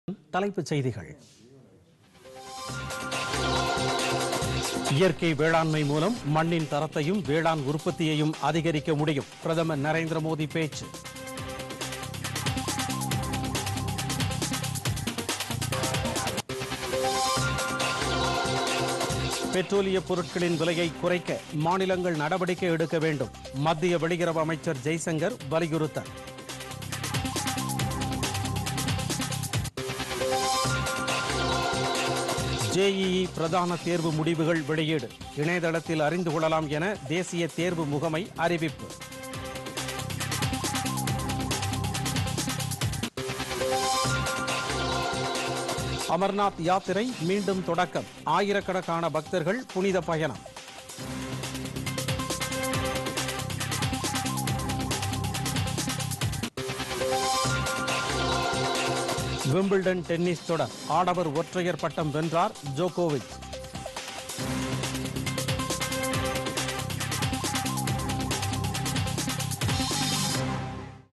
பொதிகை காலை 11.00 மணி தலைப்புச் செய்திகள் [11.07.2022]PodhigaiTamilNews
1)   இயற்கை வேளாண்மை மூலம் மண்ணின் தரத்தையும் வேளாண் உற்பத்தியையும் அதிகரிக்க முடியும் - பிரதமர் நரேந்திர மோதி பேச்சு.

2)   பெட்ரோலியப் பொருட்களின் விலையை குறைக்க மாநிலங்கள் நடவடிக்கை எடுக்க வேண்டும் - மத்திய வெளியுறவு அமைச்சர் ஜெய்சங்கர் வலியுறுத்தல்.

3)  ஜேஇஇ பிரதான தேர்வு முடிவுகள் வெளியீடு - இணைய தளத்தில் அறிந்து கொள்ளலாம் என தேசிய தேர்வு முகமை அறிவிப்பு.

4)  அமர்நாத் யாத்திரை மீண்டும் தொடக்கம் - ஆயிரக்கணக்கான பக்தர்கள்  புனிதப் பயணம்.

5)  விம்பிள்டன் டென்னிஸ் தொடர் - ஆடவர் ஒற்றையர் பட்டம் வென்றார் ஜோக்கோவிச்.